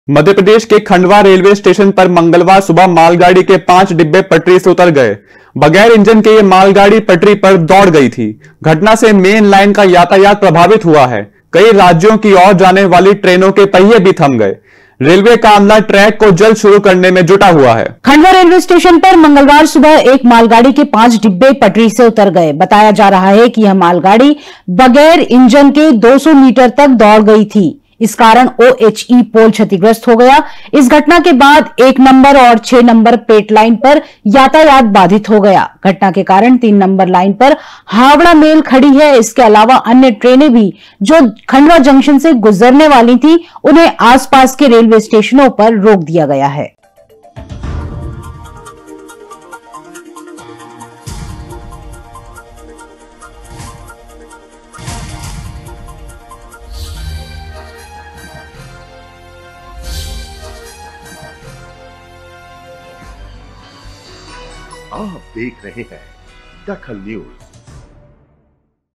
DAKHAL NEWS मालगाड़ी के 5 डिब्बे पटरी से उतरे
मालगाड़ी के 5 डिब्बे पटरी से उतरे
खंडवा रेल्वे स्टेशन पर बड़ा हादसा टल